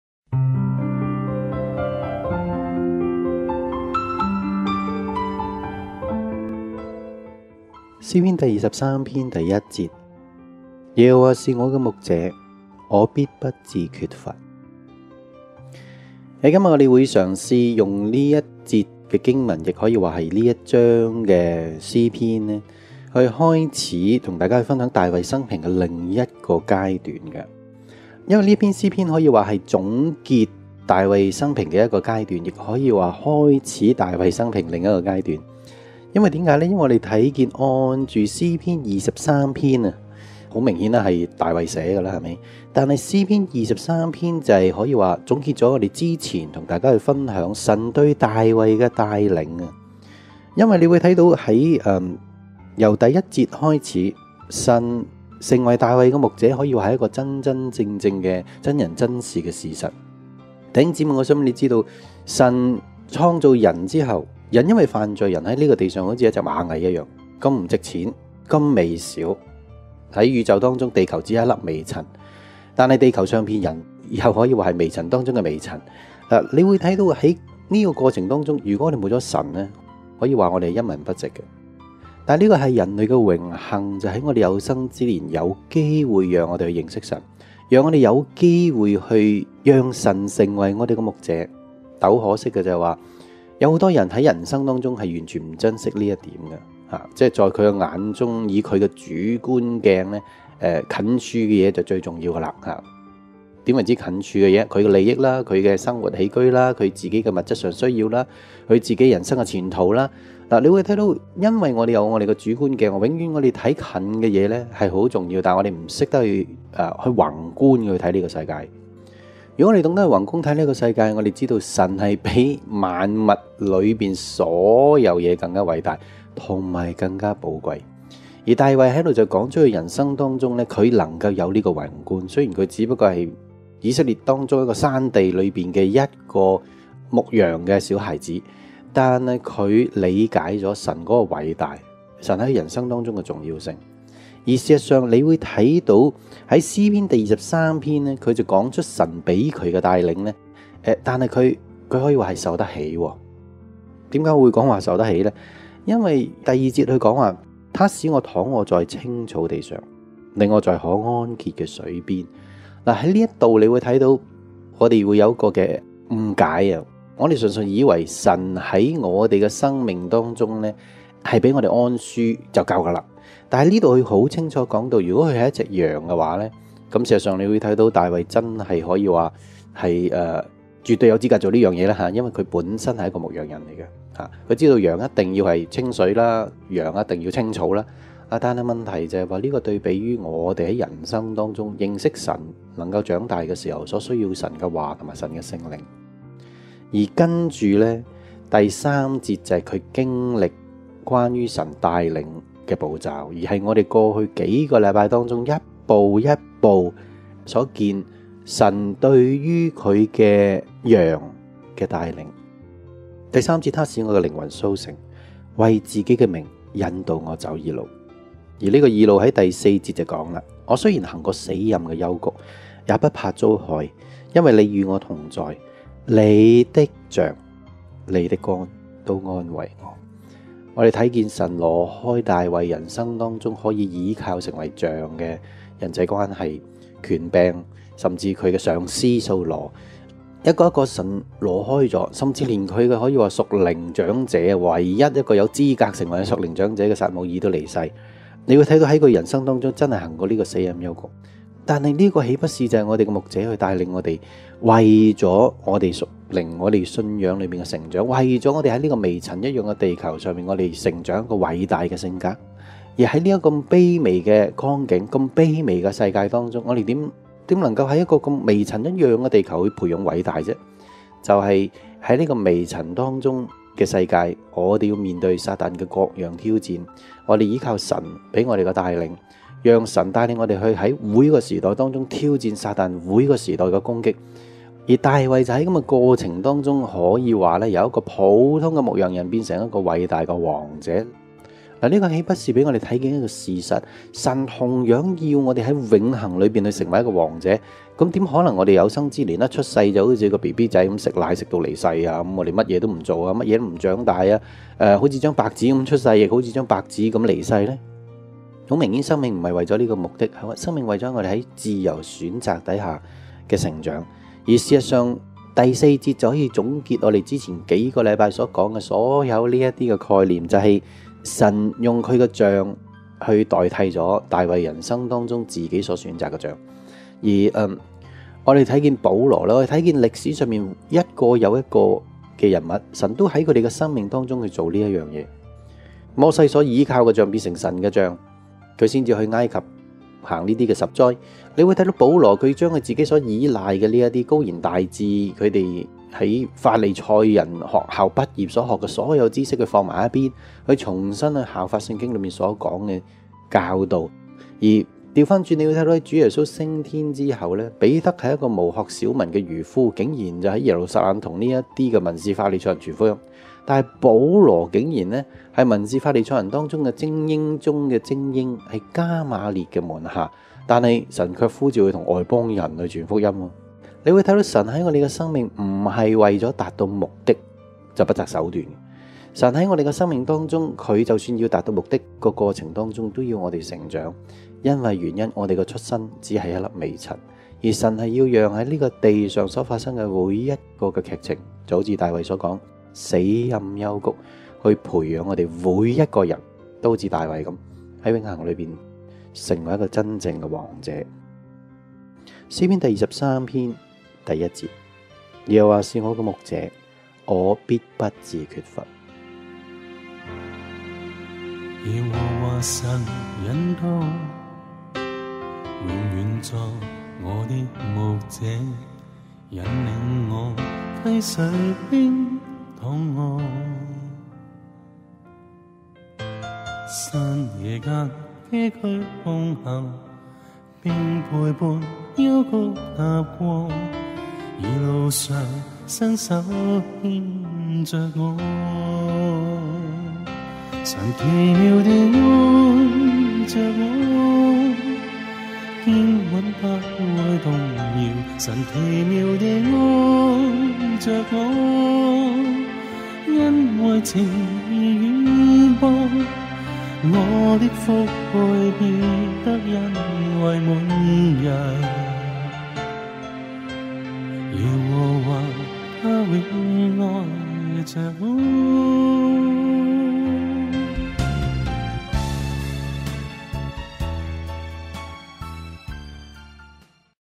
錫安教會靈修系列：栽在溪水旁 110217
詩篇 二十三篇一節：
「〔大衛的詩〕耶和華是我的牧者，我必不至缺乏。」